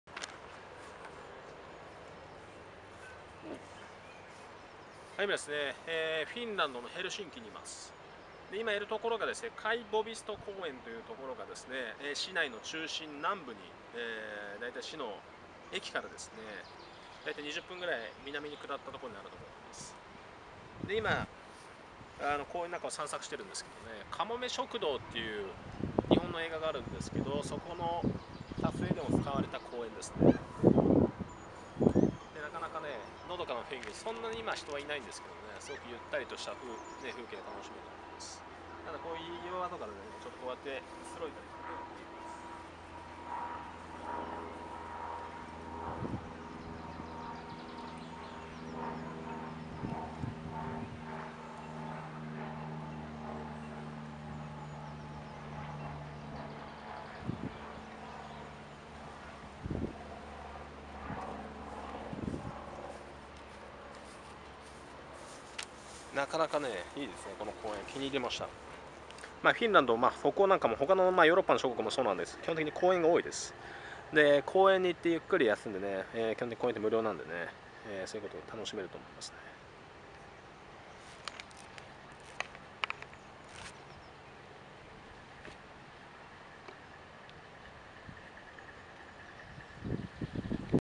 アキーラさん訪問3フィンランド・ヘルシンキ・Kaivo-puisto公園,Helsinki,Finland
フィンランドの首都ヘルシンキ（Helsinki）市街の南東、バルト海に面した港の突端に広がる公園。トラムのカイヴォプイスト（Kaivopuisto）停留所から徒歩約1分の場所にある。周辺は大使館や高級住宅街があるエリアになっている。緑の芝生が広がる広々とした園内には小道が走り、木陰にベンチが点在している。夏などには野外コンサートなども行われる。
Spread at the tip of the harbor park southeast of the city (Helsinki) Helsinki, capital of Finland, the Baltic Sea. Located 1 minute walk from the bus stop (Kaivopuisto) Kaivopuisto tram. Surrounding area has become an exclusive residential area and there are embassies. Will be running on the park trail spacious spread of green lawn, interspersed with benches in the shade of a tree. Carried out as well as open-air concerts in the summer, etc..